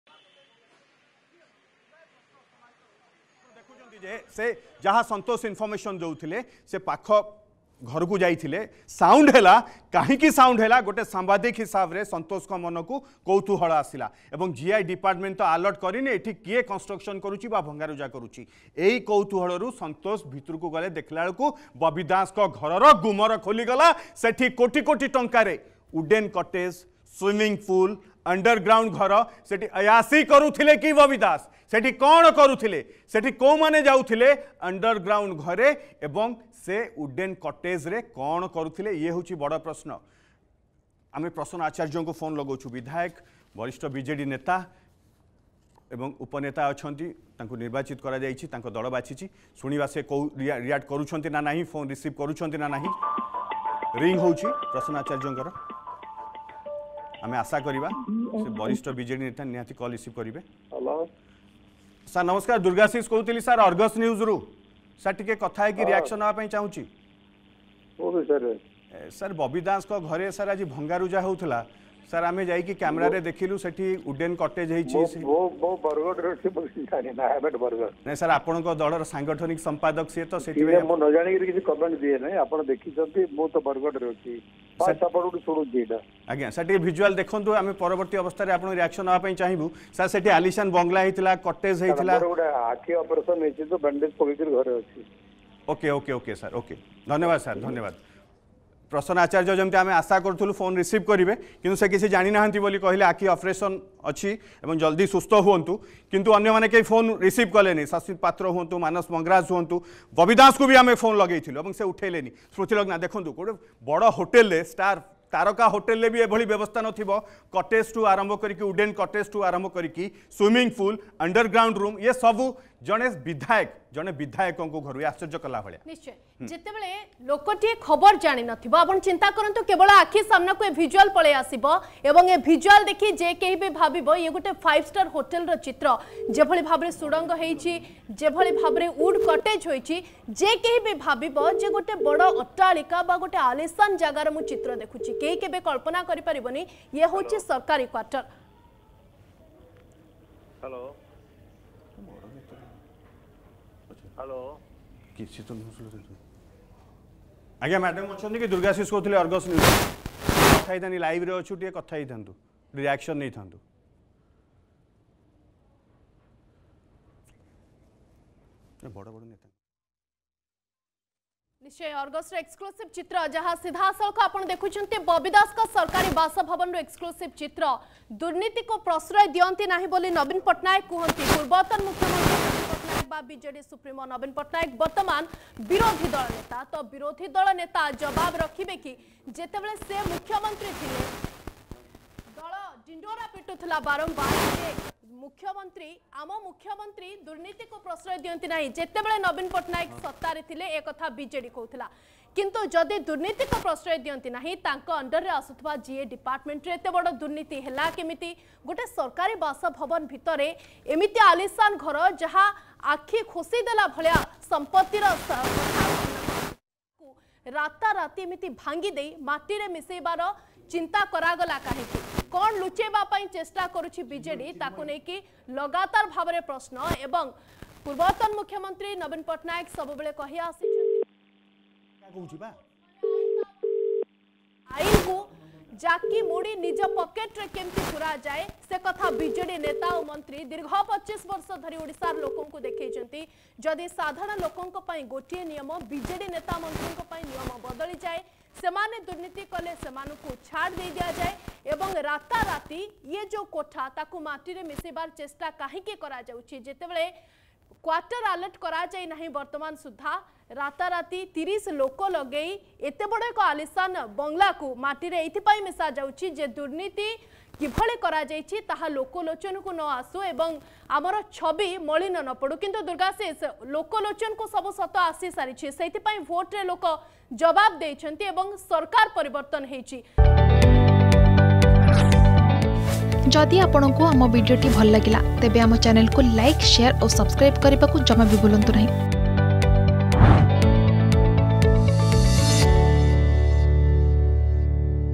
ଆଲିସାନ ବଙ୍ଗଳାରେ ଅୟାସି କରୁଥିଲେ କି ବବି ଦାସ ? || Bobby Das Govt Quarter
ସରକାରୀ କ୍ବାର୍ଟର ଅଣ୍ଡରଗ୍ରାଉଣ୍ଡ ଆଲିସାନ ବଙ୍ଗଳାରେ ଅୟାସି କରୁଥିଲେ କି ବବି ଦାସ ?